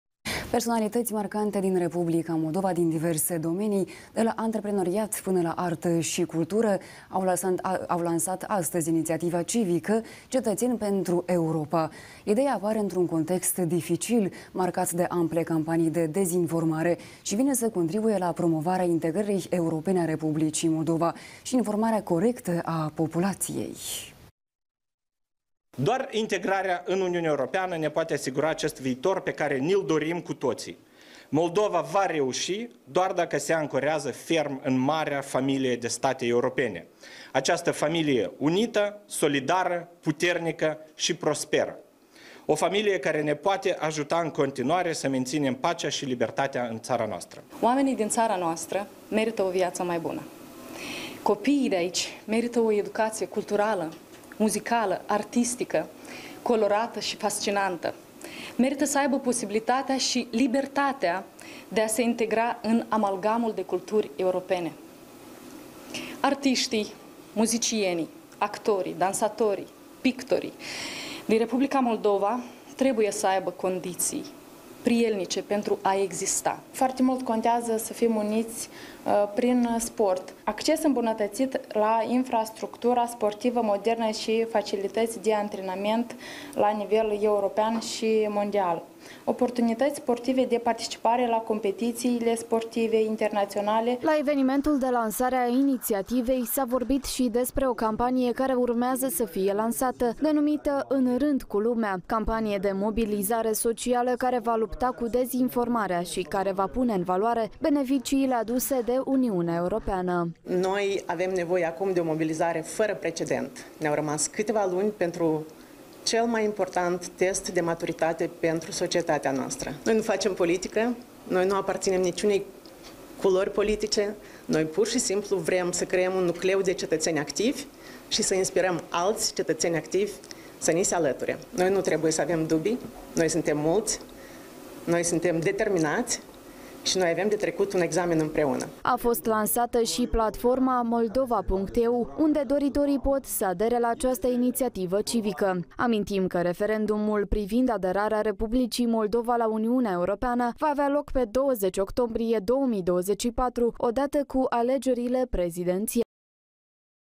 Personalităţi marcante din R. Moldova au lansat iniţiativa civică "Cetăţeni pentru Europa"
Personalităţi marcante din Republica Moldova, din diverse domenii, de la antreprenoriat până la artă şi cultură, au lansat astăzi iniţiativa civică "Cetăţeni pentru Europa". Ideea apare într-un context dificil, marcat de ample campanii de dezinformare şi vine să contribuie la promovarea integrării europene a Republicii Moldova şi informarea corectă a populaţiei.